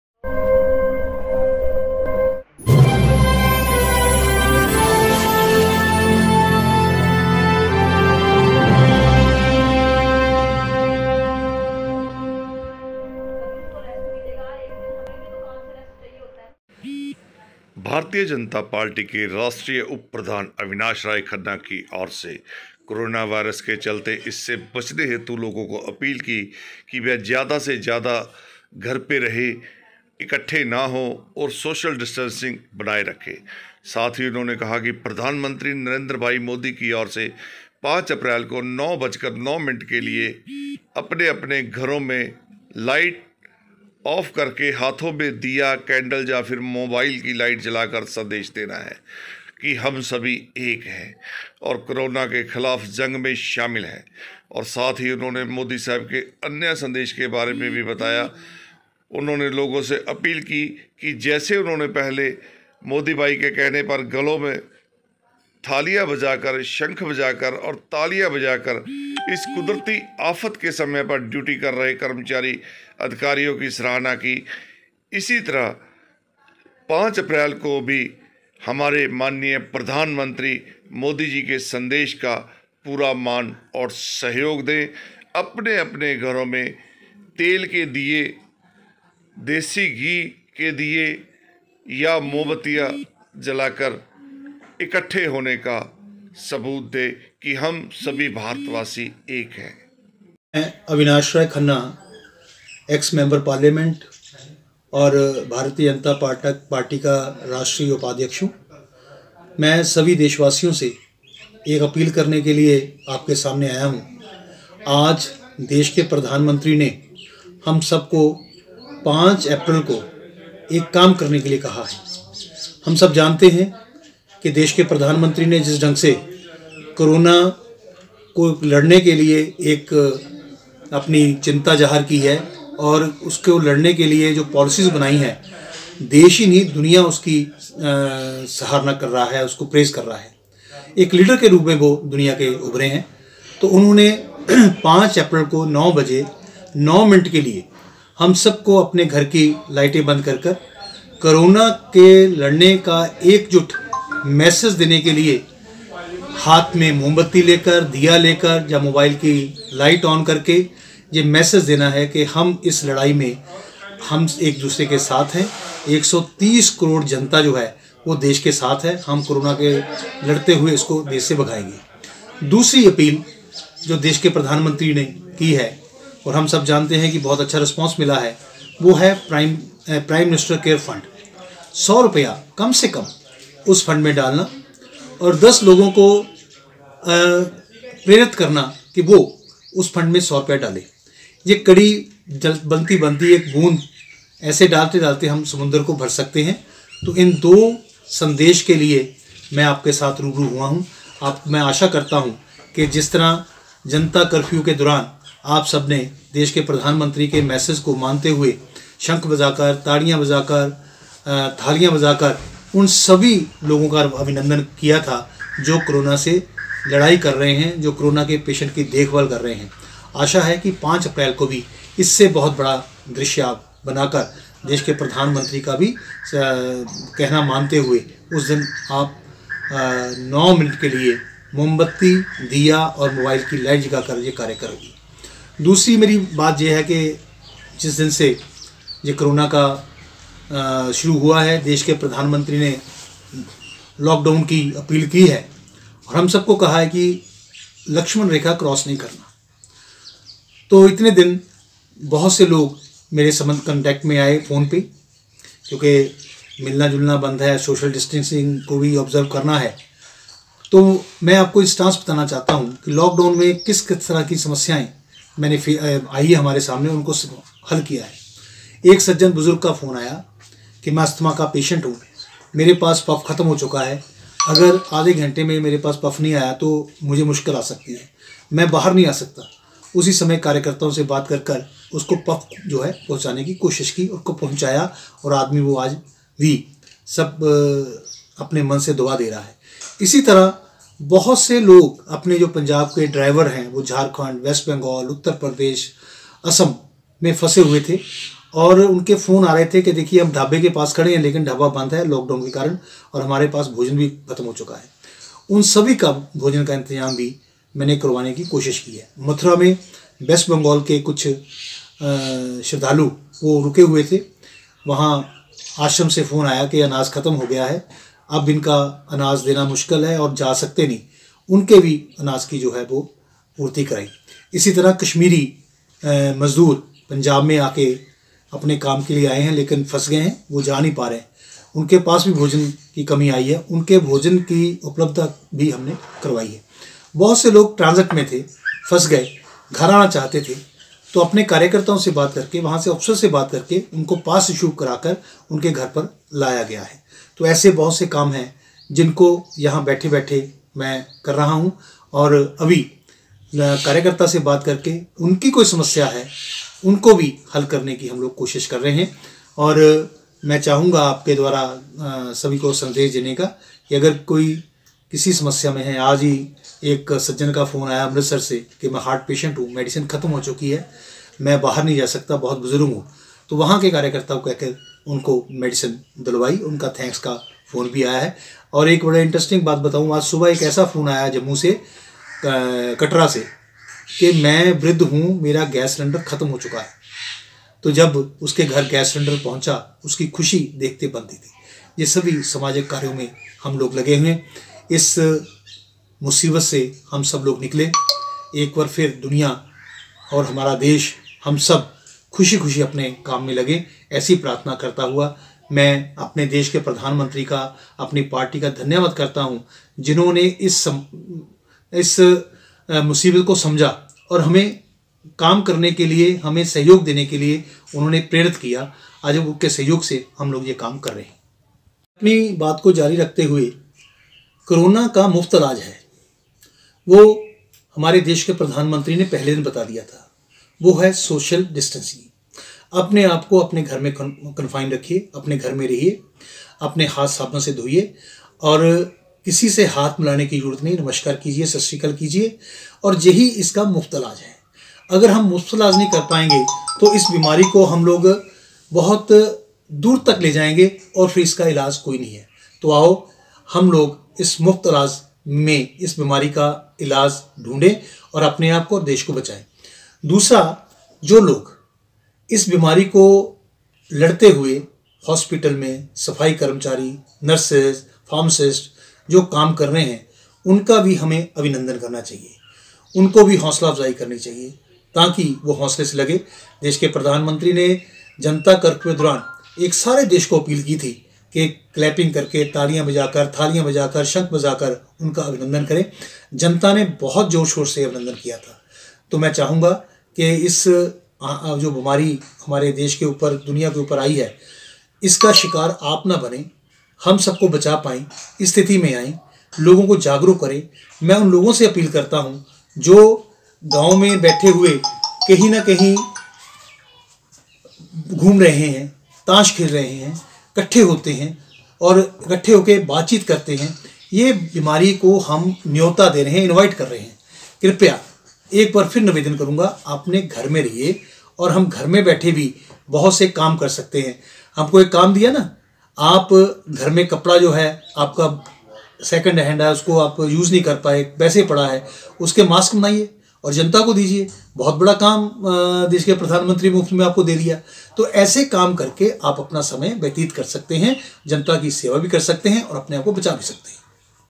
कोरोना को लेकर अविनाश राय खन्ना ने क्या कहा सुनिए
दलजीत अजनोहा
भाजपा के राष्ट्रीय उप प्रधान अविनाश राय खन्ना की और से प्रेस वार्ता दौरान इस कॅरोना वॉयरस के चलते इस से बचने हेतुं लोगो को अपील की की बह ज्यादा लोग  इकट्ठे न हो और आपस में सोशल डिस्टेंस बनाय रखे
और साथ ही उन्होंने ने प्रधानमंत्री नरिंदर भाई मोदी की और से 5 अप्रैल को 9 बजे 9 मिंट के लिये आने घरोंमें लाइट बैंड करके हाथोँ में दीया, कैंडल या फिर मोबाइल की लाइट जला कर संदेश देना है कि हम सभी एक साथ है और कॅरोना के खिलाफ जंग में शामिल है और साथ ही उन्होंने मोदी साहिब के अन्य संदेश के बारे में भी बताया उन्होंने लोगो से अपील की की जैसे उन्होंने पहले मोदी साहिब कहने पर घरों में थालिया बजा  कर,शंख बजा कर और तालिया बजाकर इस क़ुदरती आफत के समय पर डयूटी कर रहे कर्मचारियों अधिकारियों का सराहना की इसी तरह 5 अप्रैल को भी मोदी साहिब के संदेश को मान कर पूरण सहयोग दे